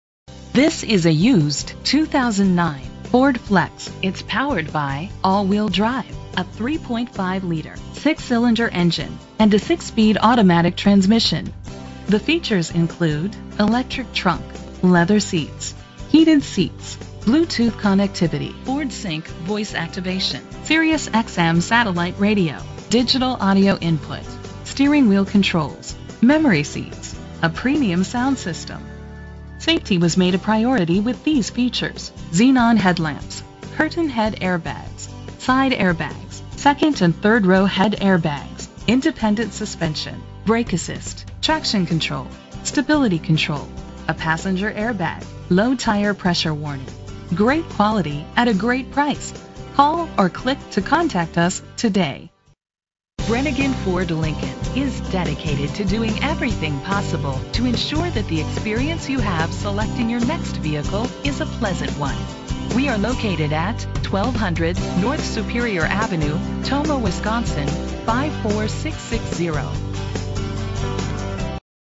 2009 Ford Flex - Tomah WI
Phone:
Year: 2009
Make: Ford
Model: Flex
Trim: Limited
Engine: 3.5 liter 6 cylinder 24 valve
Transmission: 6-Speed Automatic
Color: Silver
Mileage: 85680
Address:
1200 North Superior Avenue